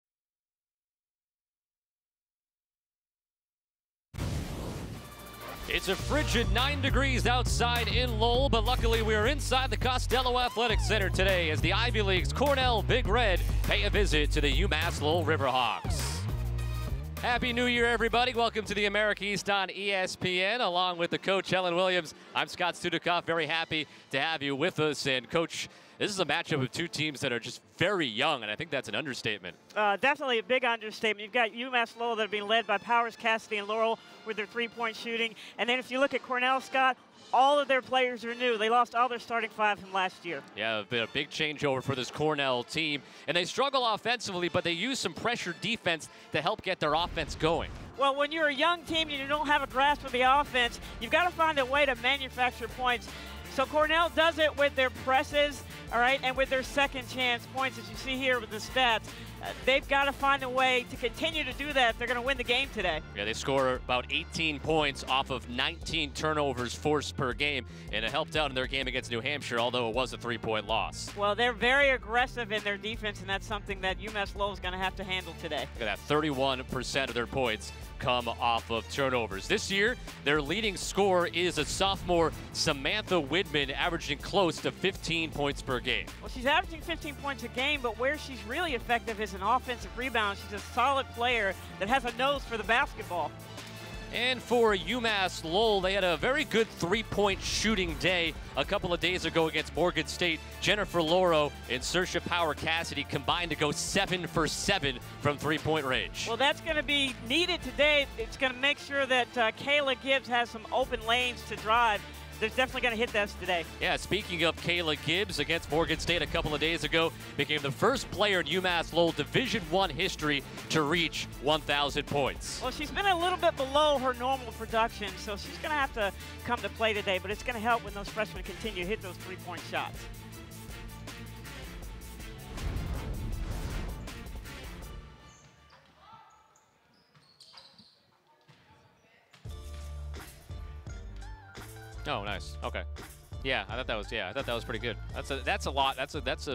ESPN3 Women's Basketball | UMass Lowell vs Cornell Open
Game open from ESPN3 broadcast on Dec. 31, 2017